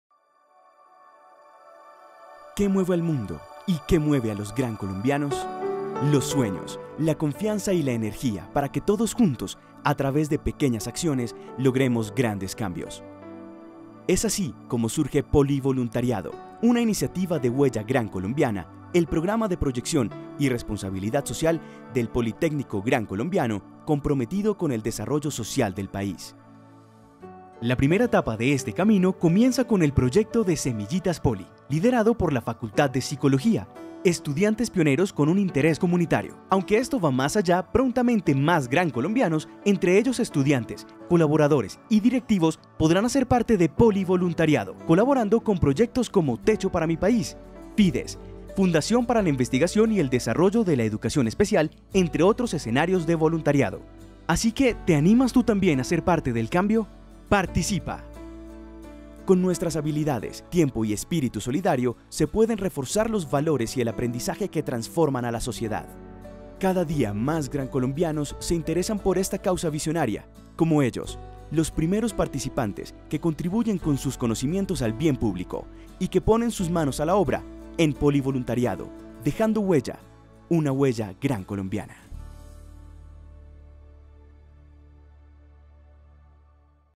Una pequeña acción para un gran cambio | Polivoluntariado
Polivoluntariado es una iniciativa de nuestro programa de responsabilidad social Huella Grancolombiana en alianza con la organización TECHO,  y que se propone contribuir al cambio social con el apoyo de toda la familia grancolombiana.